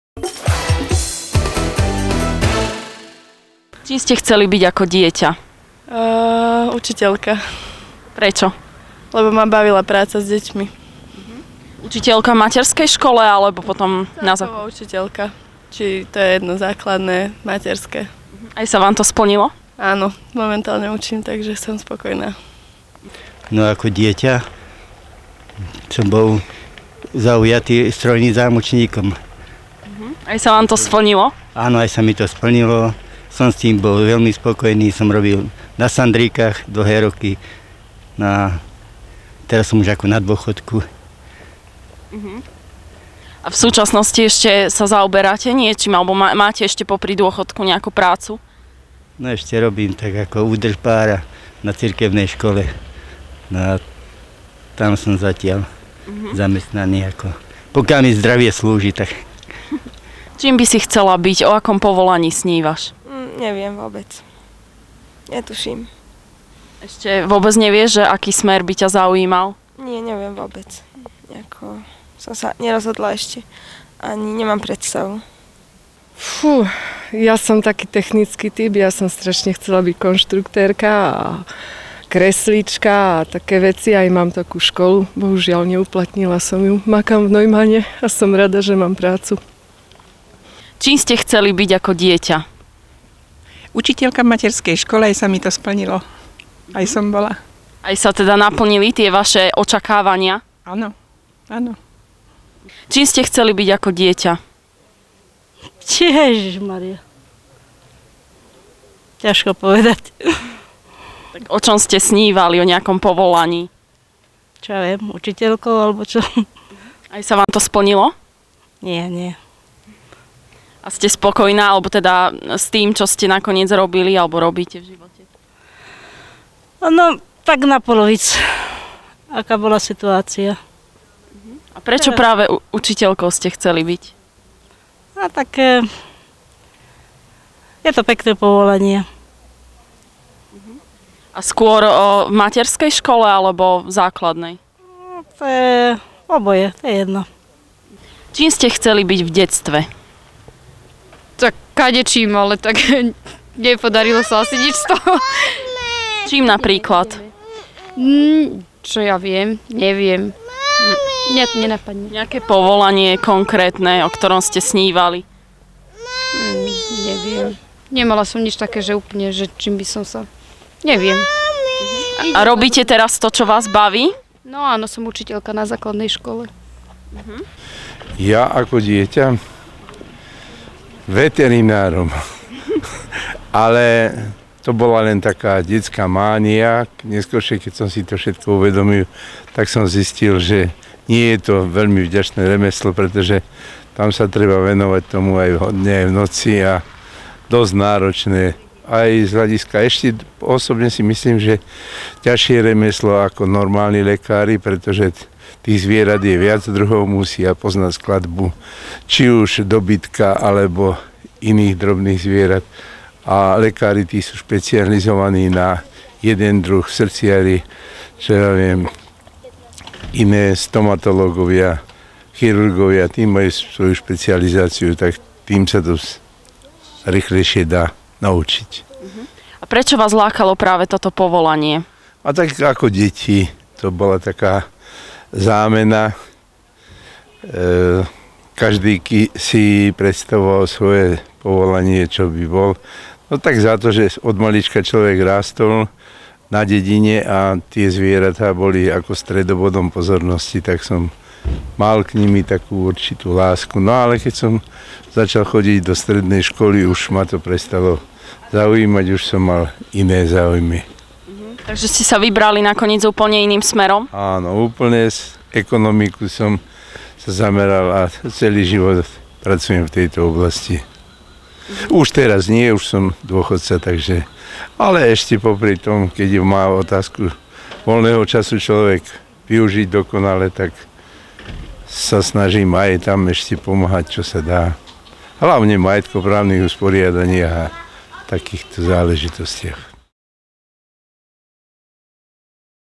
Čím ste chceli byť?
Anketa v uliciach mesta Žarnovica s náhodnými chodcami na tému spomienok z detstva budúceho povolania.